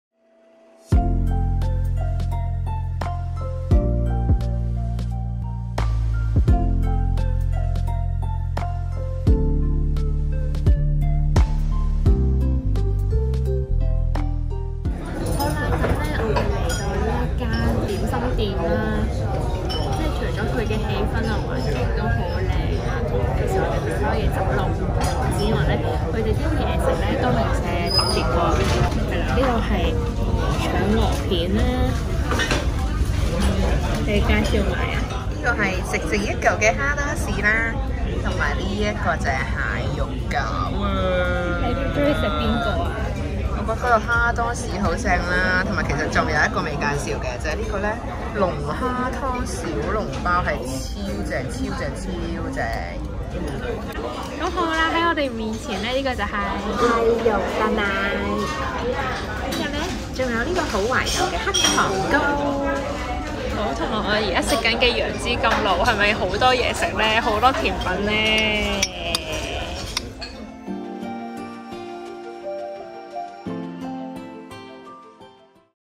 ［慢活房子🏠-收爐啦喂食餐好 一盅兩件唔老土］九号館 NOVE Chinese Kitchen/米芝蓮/中環
去到商業區中環想飲茶，有冇好介紹先？唔想係食嚟食去嗰幾間？小編們發現咗一間位於利源東街嘅隱世點心店，由米芝蓮點心老師傅黃耀波主理，食物即叫即煮，味道超晒班，加上中西合璧的裝修設計，完全唔似一般酒樓... 不過間店好full，真心要book定位先好去呀♥️
九号館
地址:中環利源東街9號地舖